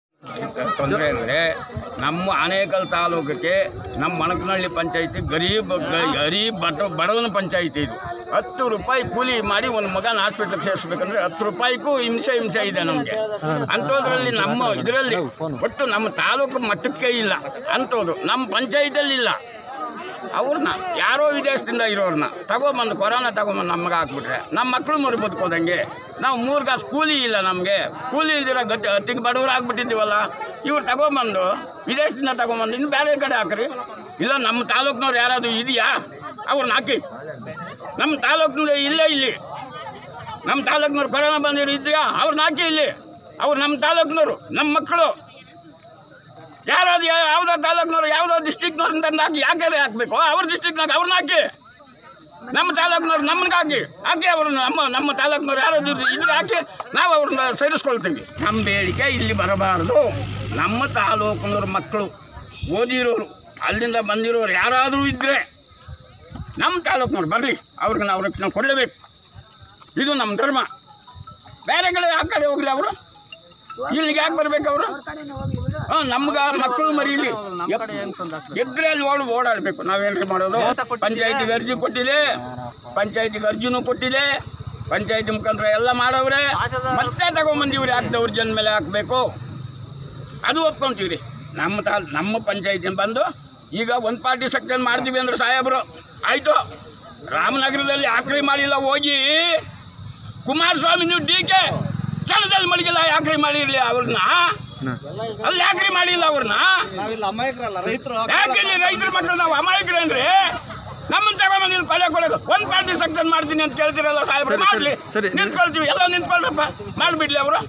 ವಸತಿ ಶಾಲೆಯಲ್ಲಿ ಕ್ವಾರೆಂಟೈನ್ ಮಾಡಲು ಗ್ರಾಮಸ್ಥರಿಂದ ತೀವ್ರ ವಿರೋಧ
ಆನೇಕಲ್ : ವಸತಿ ಶಾಲೆಯಲ್ಲಿ ಕ್ವಾರೆಂಟೈನ್ ಮಾಡಲು ಗ್ರಾಮಸ್ಥರು ತೀವ್ರ ವಿರೋಧ ವ್ಯಕ್ತಪಡಿಸಿರುವ ಘಟನೆ ಬೆಂಗಳೂರು ಹೊರವಲಯ ಆನೇಕಲ್ ತಾಲೂಕಿನ ವಣಕನಹಳ್ಳಿ ಗ್ರಾಮ ಪಂಚಾಯತಿ ವ್ಯಾಪ್ತಿಯ ತೆಲಗರಹಳ್ಳಿಯ ಕಿತ್ತೂರು ರಾಣಿ ಚೆನ್ನಮ್ಮ ವಸತಿ ಶಾಲೆಯ ಬಳಿ ನಡೆದಿದೆ